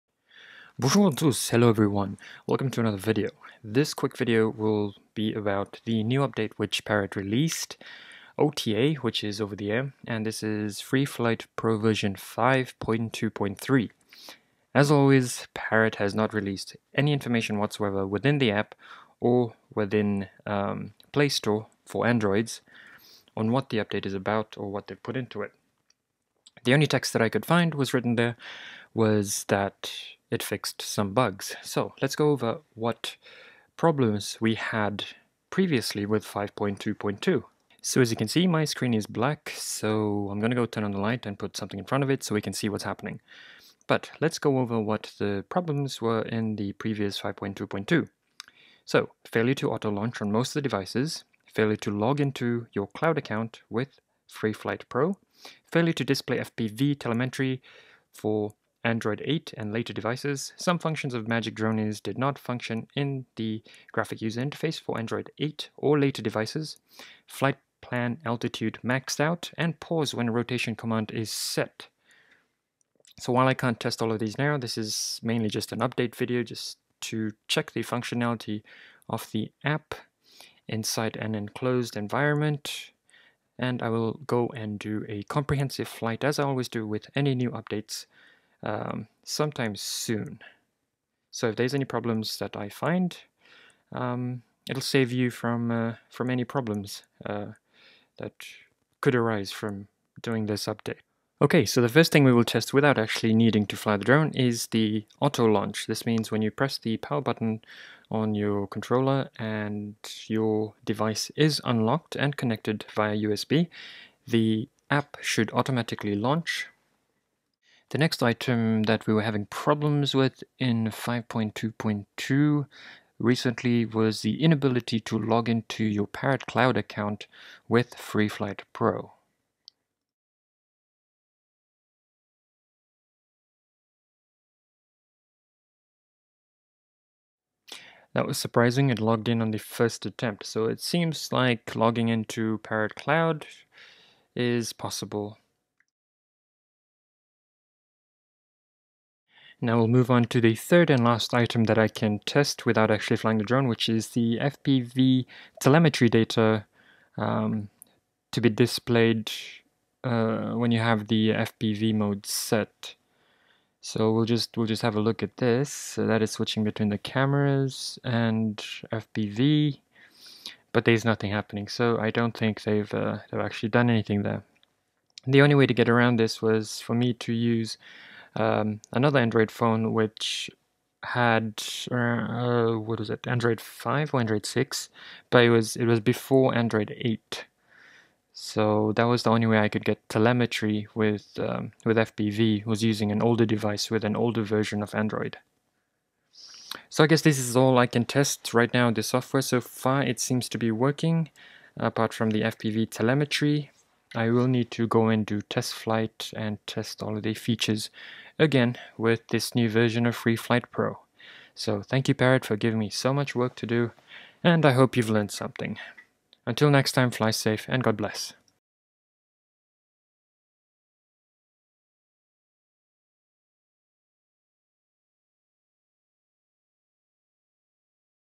VLOG014 | FreeFlight Pro 5.2.3 | Quick talk
FreeFlight Pro 5.2.3 update for androids. No information about it as of yet. If someone has the change-log please are it in the comments so I can target my testing to those specific changes. Apart from that its testing of known problems from 5.2.2 than a comprehensive flight and testing of all common features.

EXTRA VIDEOS, GEAR AND LINKS

-VIDEO STUFF-
BEBOP 2 | Tutorials | Overview |

-Parrot STUFF-
BEBOP 2 Case

-DJI STUFF-
DJI Drones